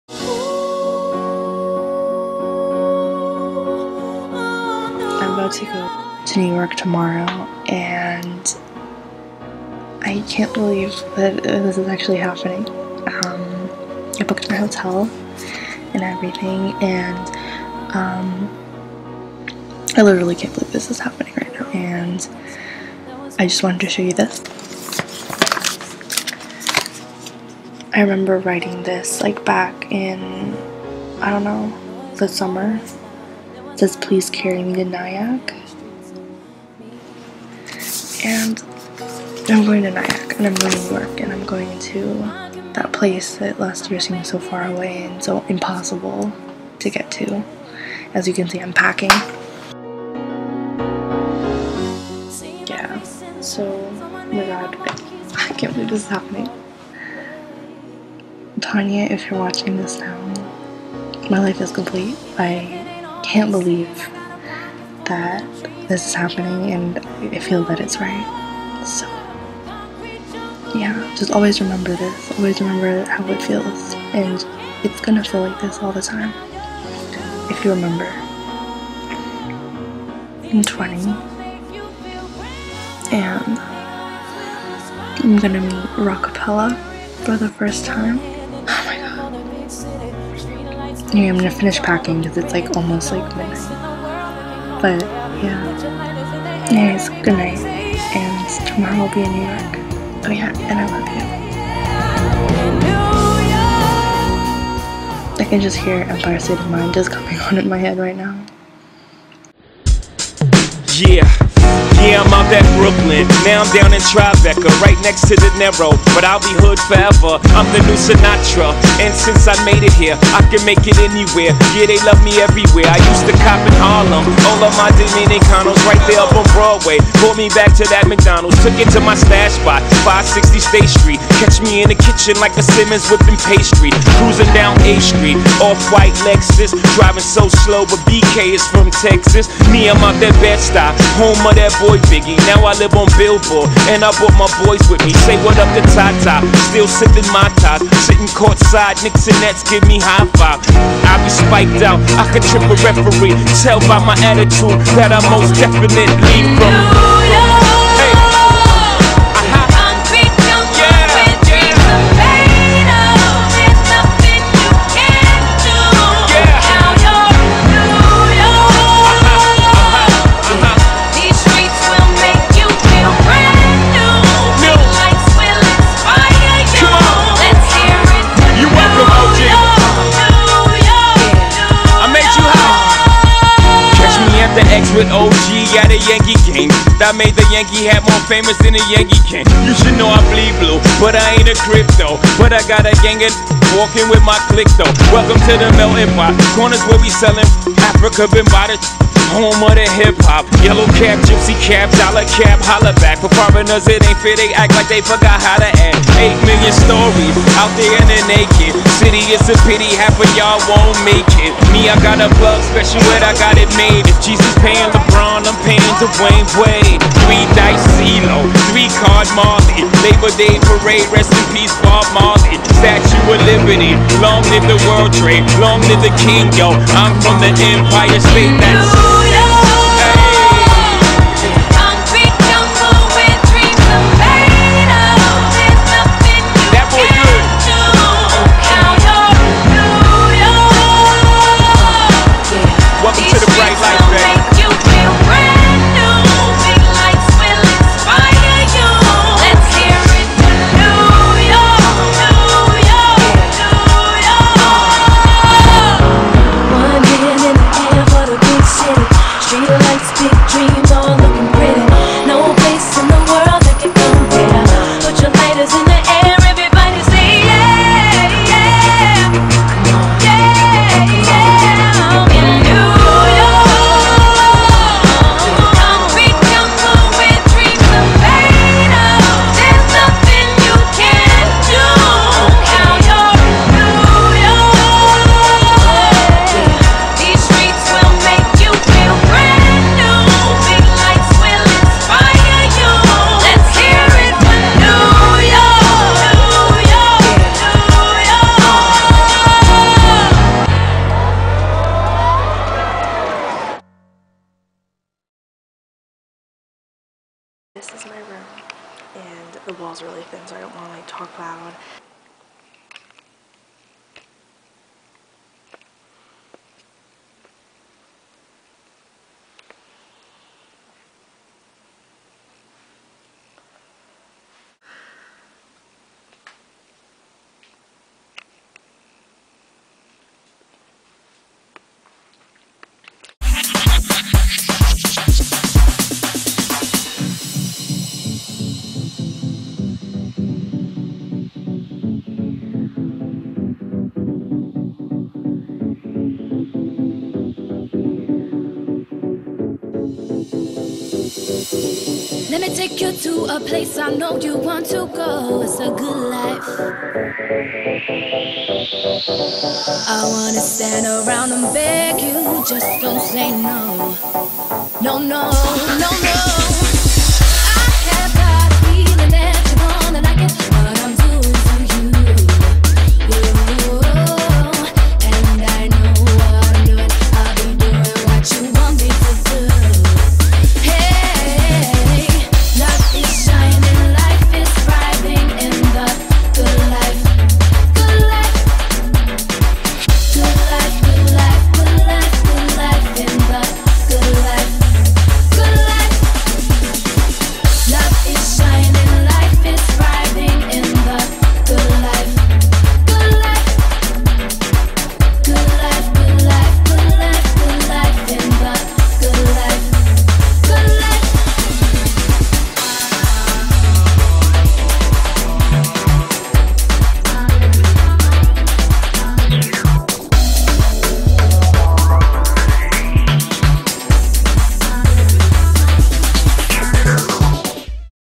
♡ NYC Diaries: Day 1 - Getting There!/Toronto meets NYC/Times Square!
I went to NYC in the winter to meet Rockapella/explore the city of my dreams! If you didn't know from my Tumblr/Twitter, its been my dream to go to New York for the longest time!

XO
Tanya

(btw I had to re-upload this bc the sound wasn't working)